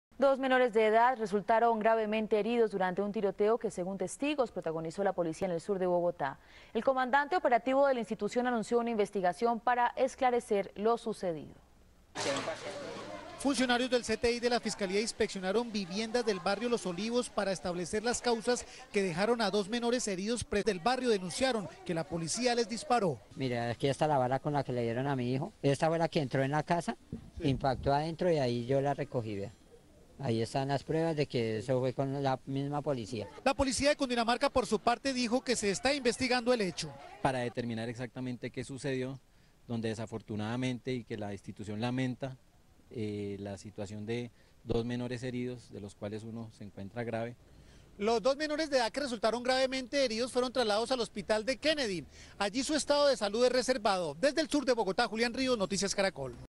Culpan a policías de tiroteo que dejó a dos jóvenes heridos - Enero 11 de 2013
Los hechos se registraron durante un operativo en el barrio Los Olivos, en el sur de Bogotá. Enero 11 de 2013.